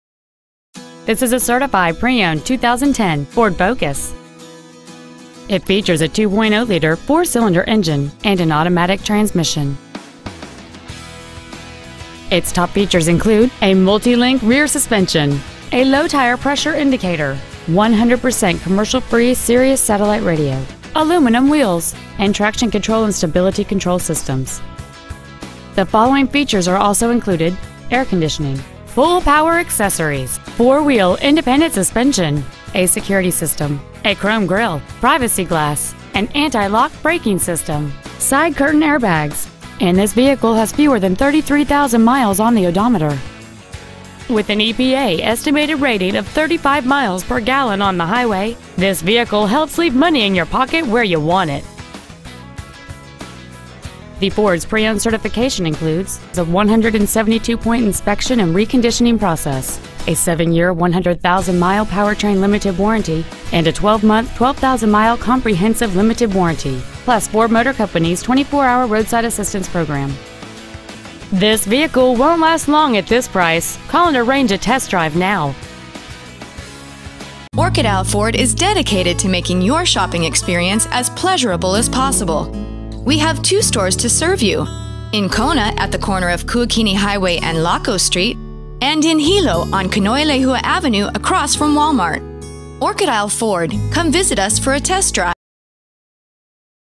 2010 Ford Focus Certified Hilo HI
We have been honored to serve the Hilo HI area , we promise that your experience at our dealership will exceed your expectations !

 Year : 2010
 Make : Ford
 Model : Focus
 Engine : 2.0L I4 16V MPFI DOHC
 Trans . : AUTOMATIC
 Exterior : SILVER
 Miles : 32,939

 Stock : 36563A1